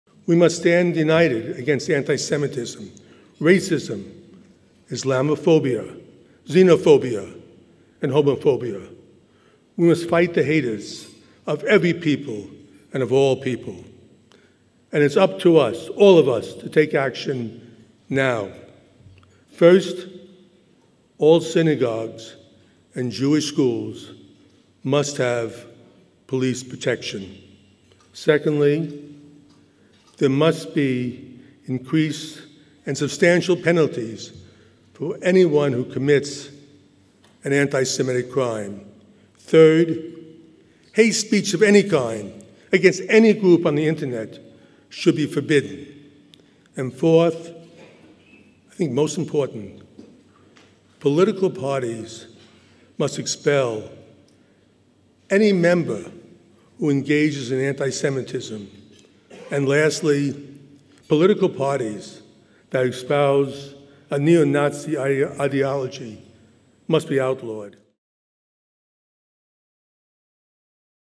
WJC President Ronald S. Lauder: We must stand united against antisemitism
The ancient hate against the Jews is rearing its ugly head again all over Europe. Now we need action, not words. We must stand united against antisemitism, racism, islamophobia, xenophobia, and homophobia.

World Jewish Congress President Ronald S. Lauder laid out steps that all governments must take to protect their Jewish communities.